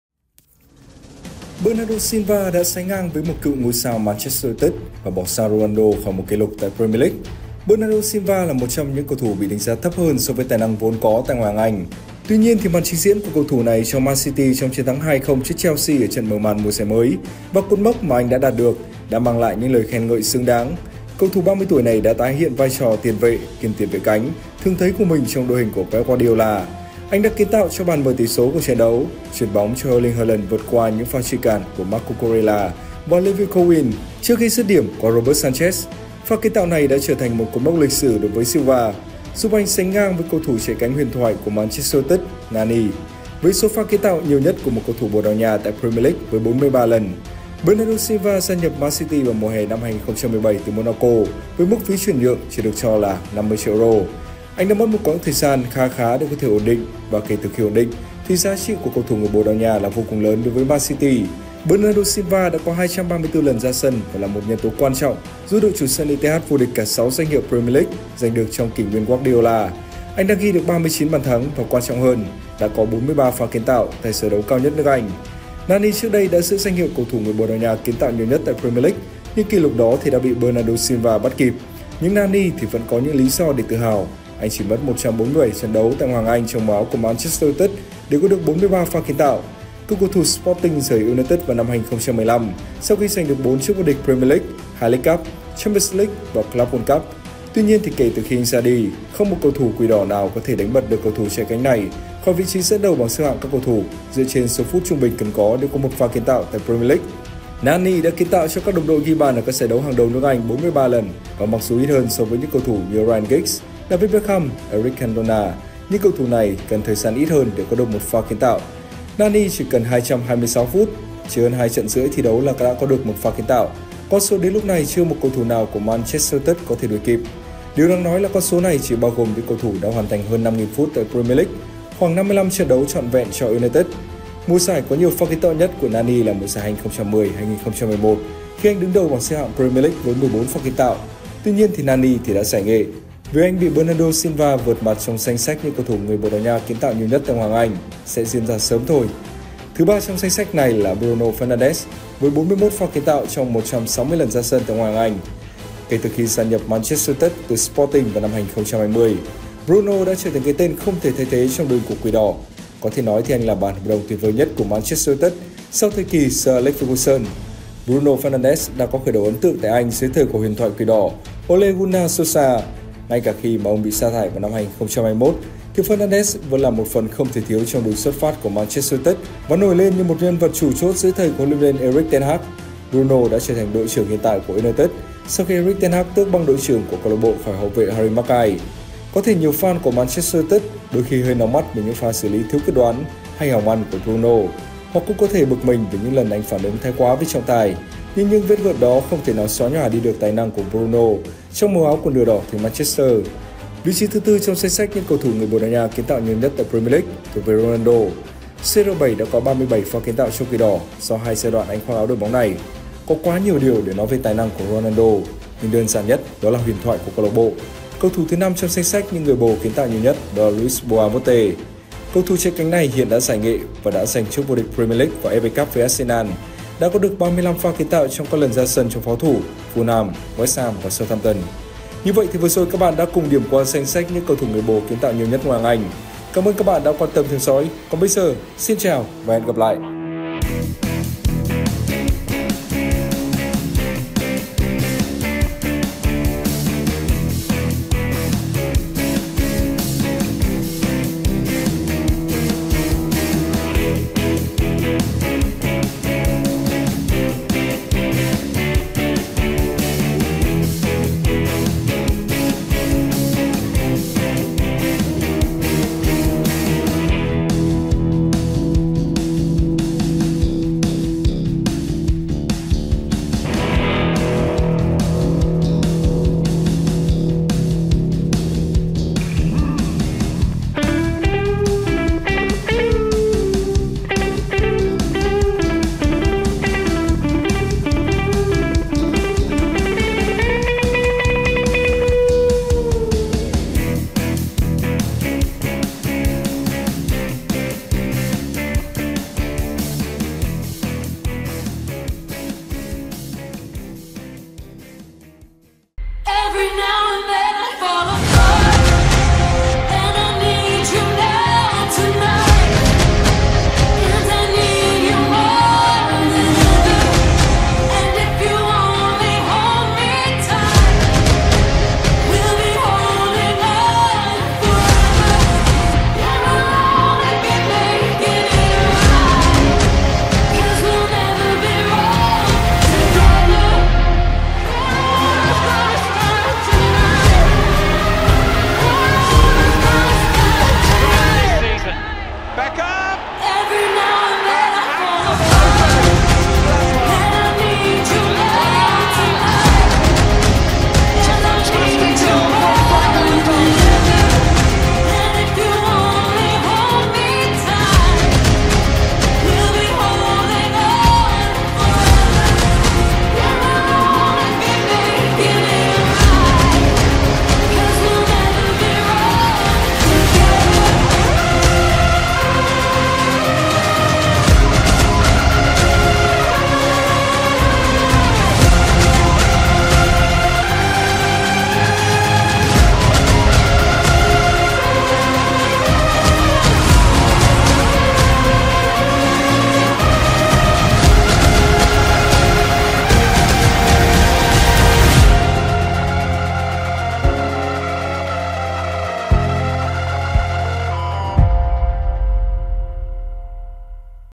TOP 5 CẦU THỦ BỒ ĐÀO NHA KIẾN TẠO NHIỀU NHẤT EPL: BERNARDO SILVA LÀM NÊN LỊCH SỬ - RONALDO ĐẮNG CẤP
Góc nhìn sân cỏ: Top 5 cầu thủ Bồ Đào Nha kiến tạo nhiều nhất EPL: Bernardo Silva làm nên lịch sử - Ronaldo đẳng cấp | Ngoại Hạng Anh 24/25